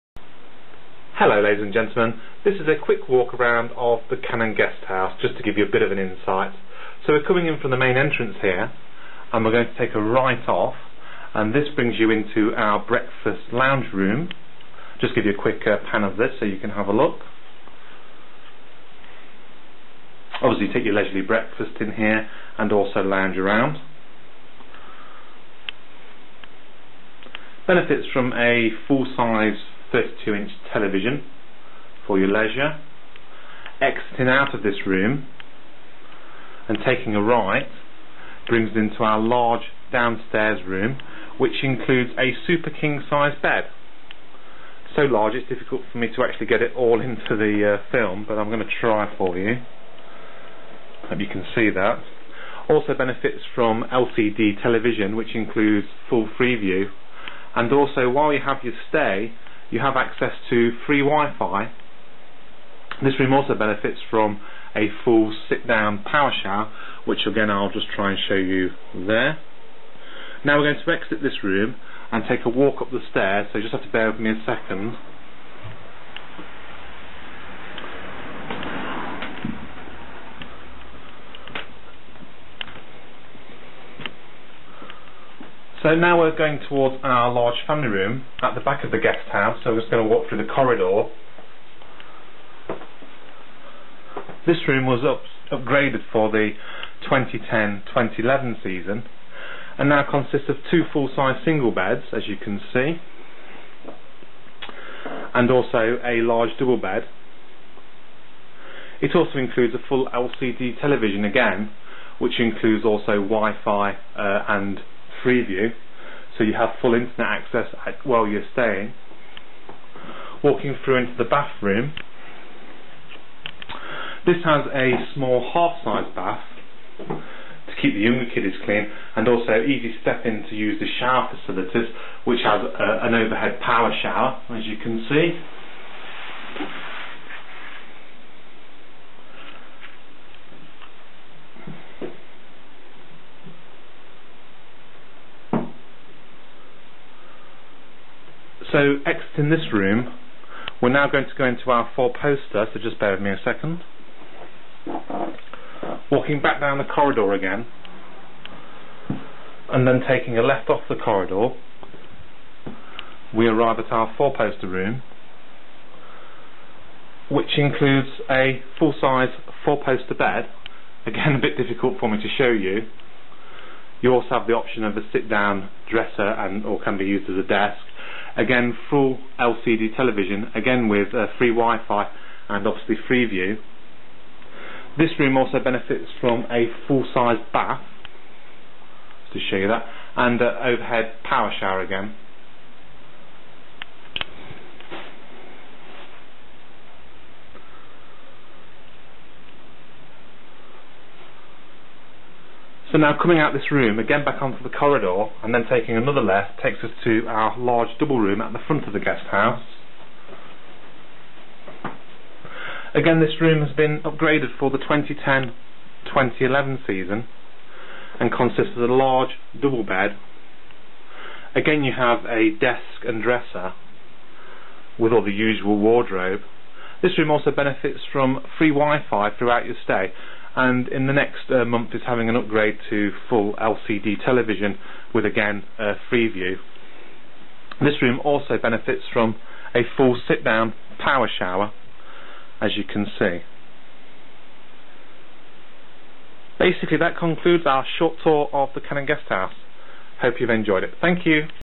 The Cannon Guest House
A Short Video Tour of the Cannon Guest House Mablethorpe